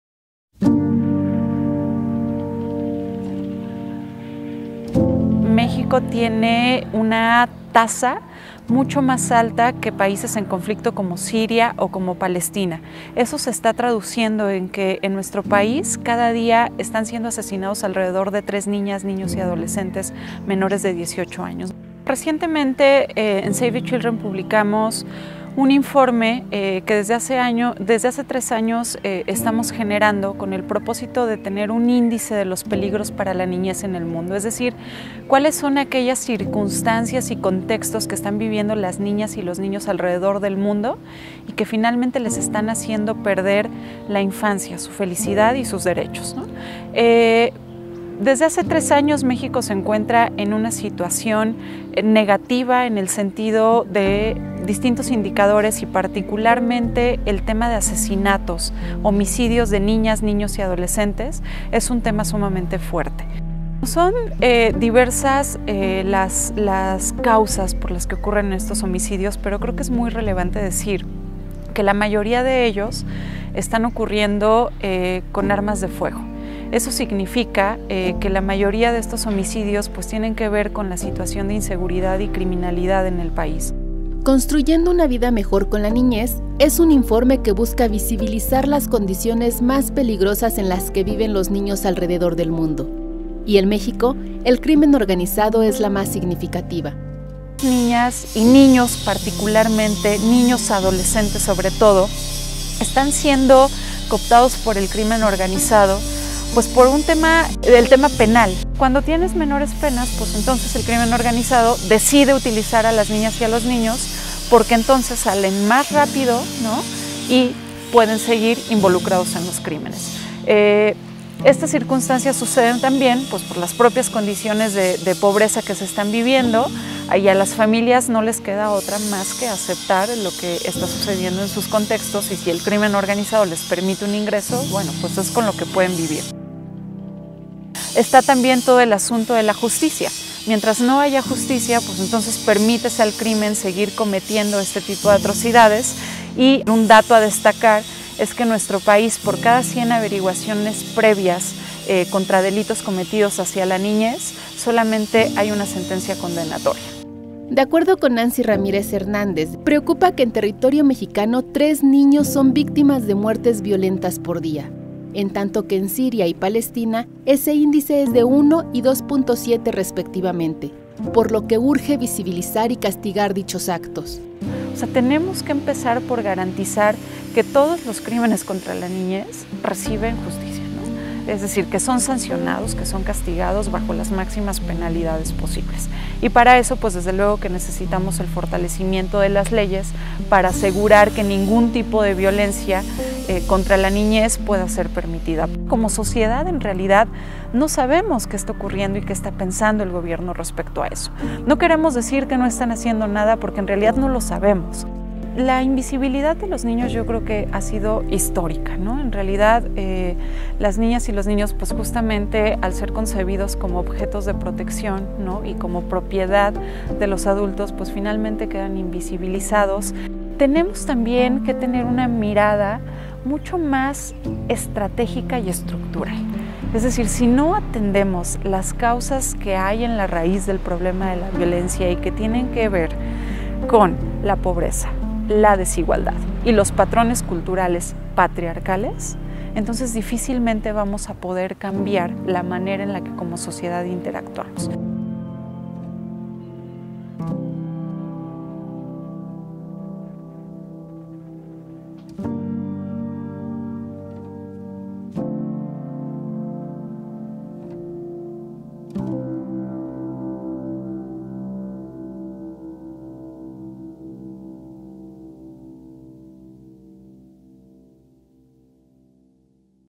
Cada día son asesinados tres menores en México: Save The Children
A partir de 2006 se incrementó el asesinato de niños mexicanos; mueren tres menores cada día.